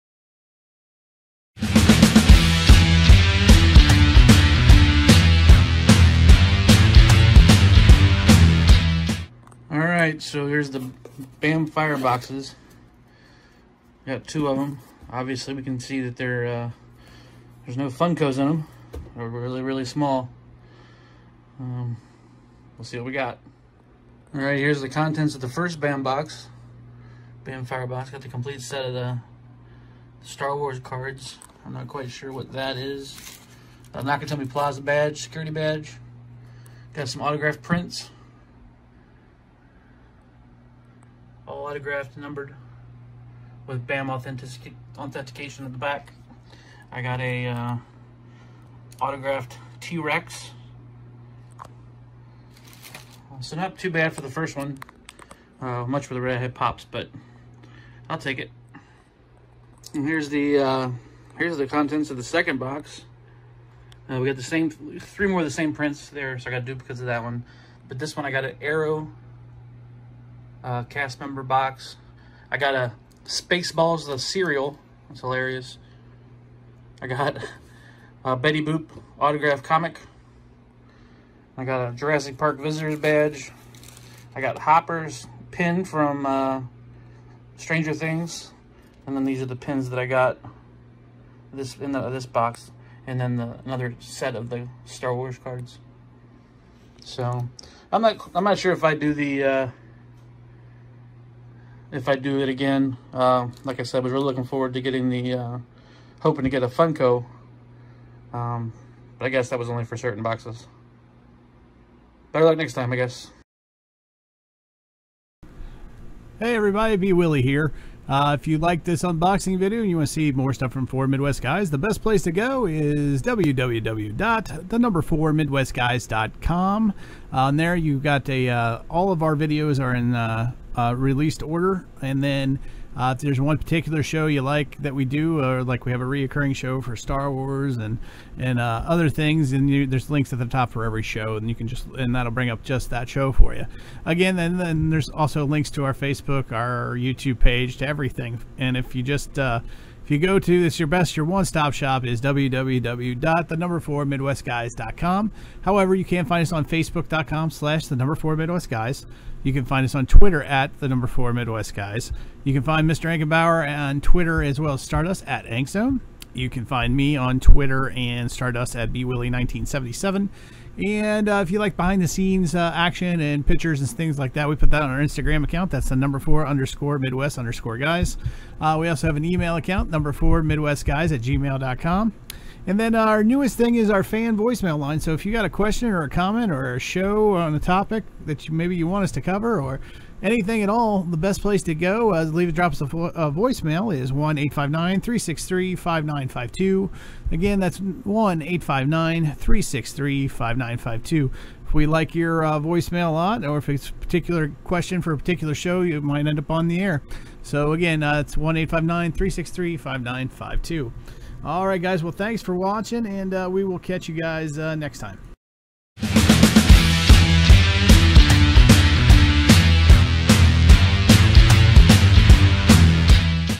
BAM FIREBOX UNBOXING
Join Mr Ankenbauer as he unboxes 2 BAM FIREBOXES. What mysterious and wonderful prizes will he get? Tune in to find out!